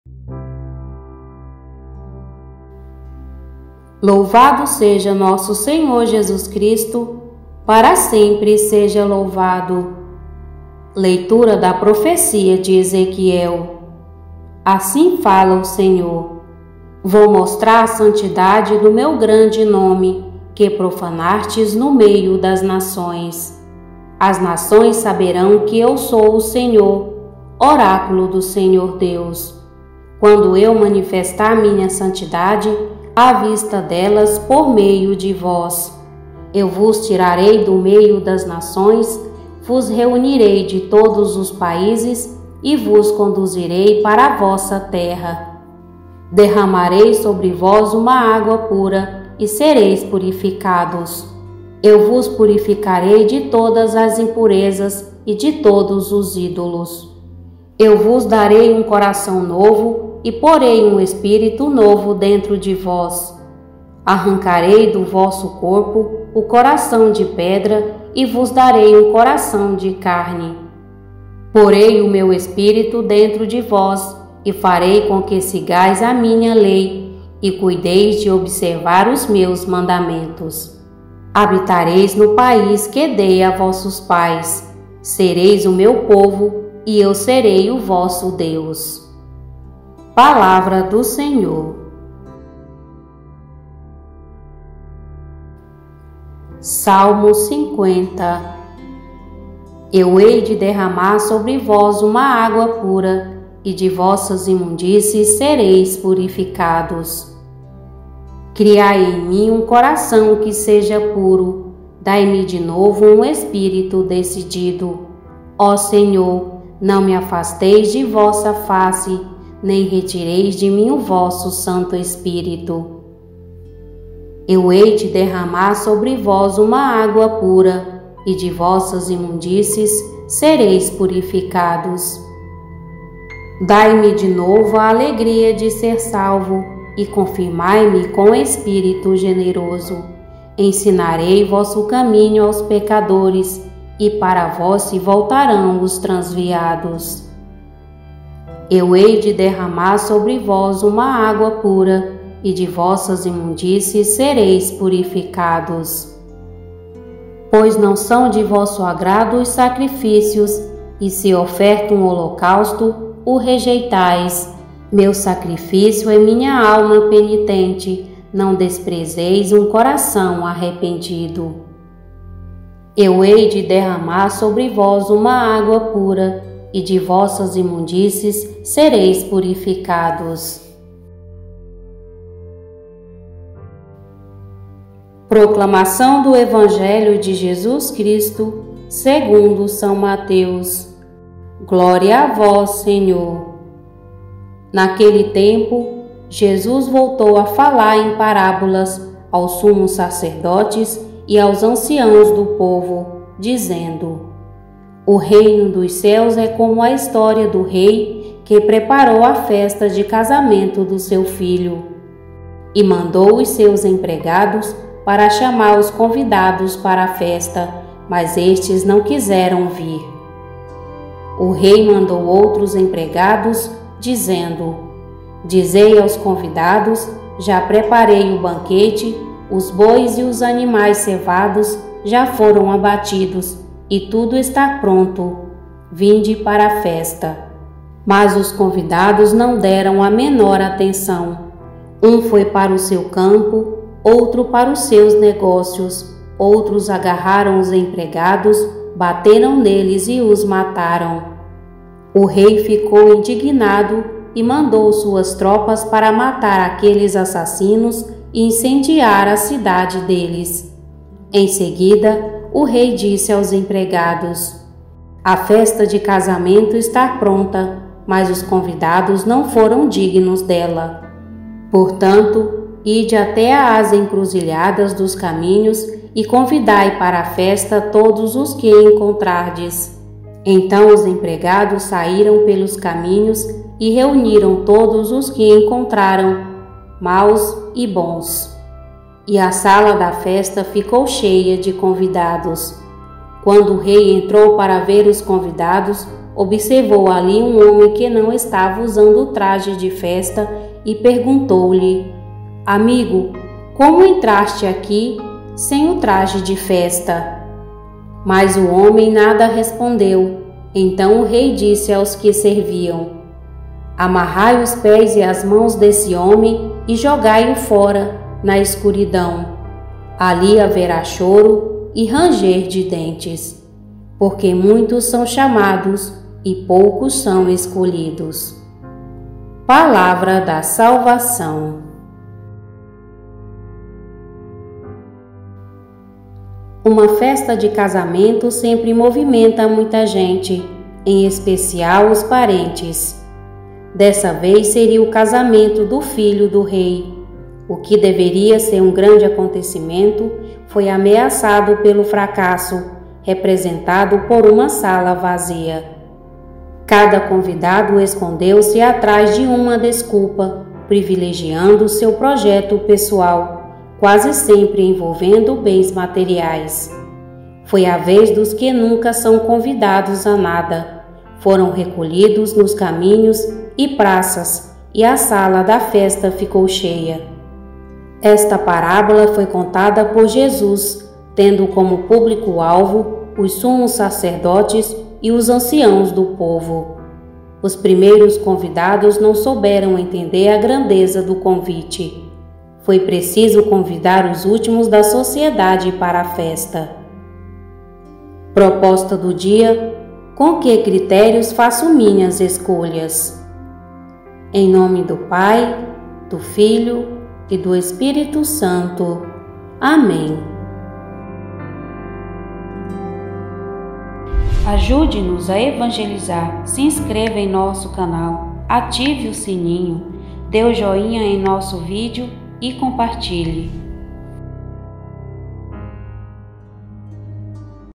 LITURGIA DO DIA 20 DE AGOSTO - Narrada com Reflexão - Quinta-feira da 20ª Semana Do Tempo Comum
Liturgia Católica Narrada com Reflexão do Evangelho
1ª Leitura - Ez 36,23-28
Salmo - Sl 50,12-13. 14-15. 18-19 (R. Ez 36,25)
Evangelho - Mt 22,1-14 - "Convidai para a festa todos os que encontrardes."
Reflexão do Evangelho

Amados irmãos, diariamente o Canal Refletindo a Vida publica a liturgia diária logo pela manhã.
Medite a palavra de Deus conosco.